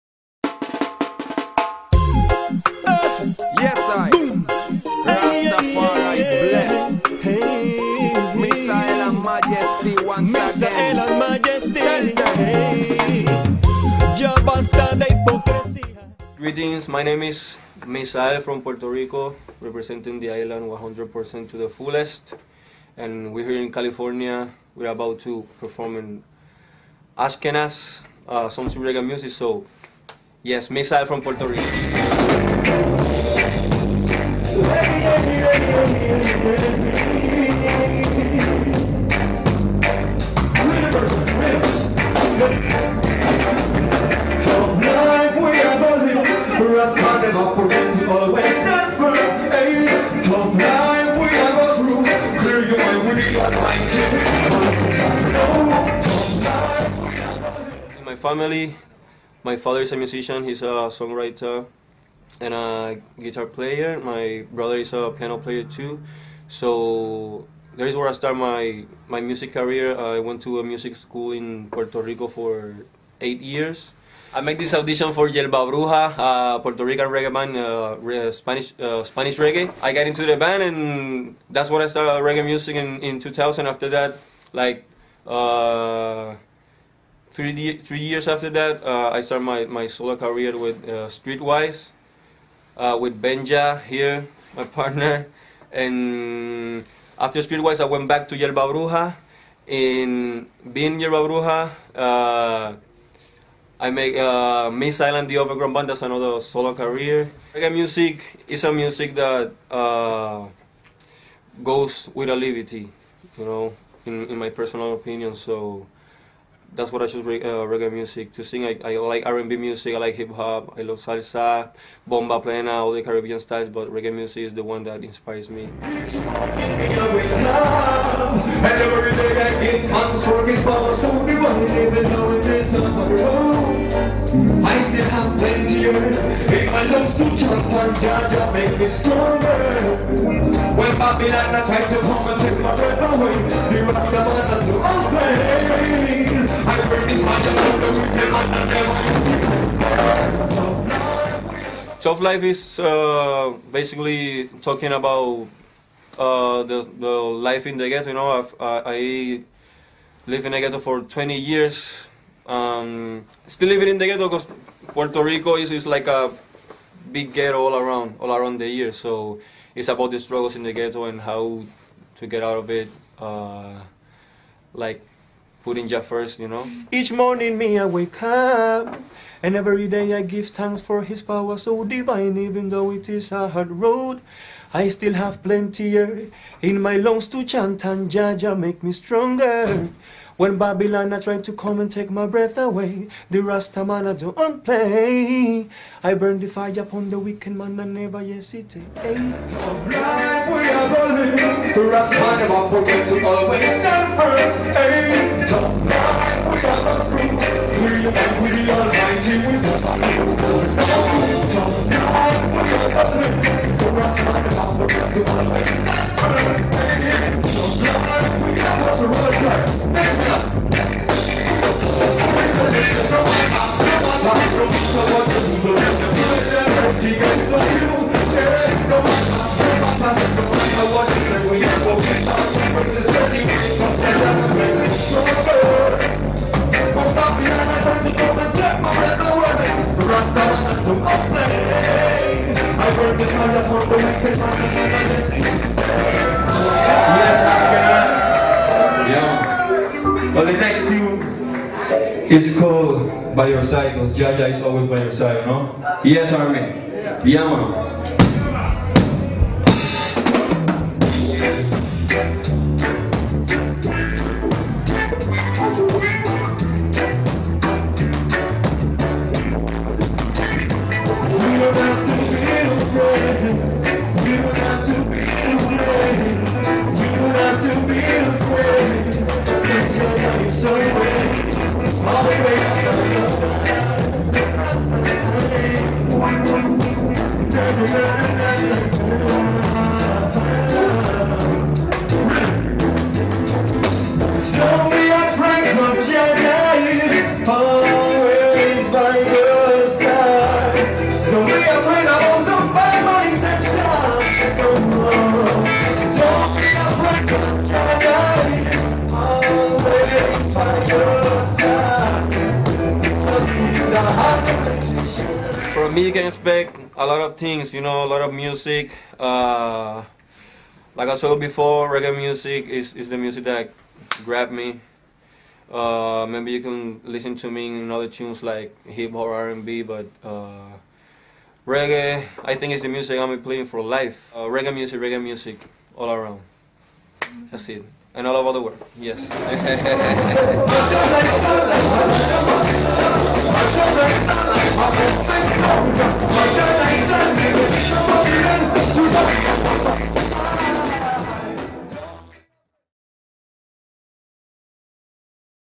Interview with Reggae Artist from Puerto Rico MISAEL
Yemaya Pictures and Iration Time Media presents interview with reggae artist from Puerto Rico MISAEL.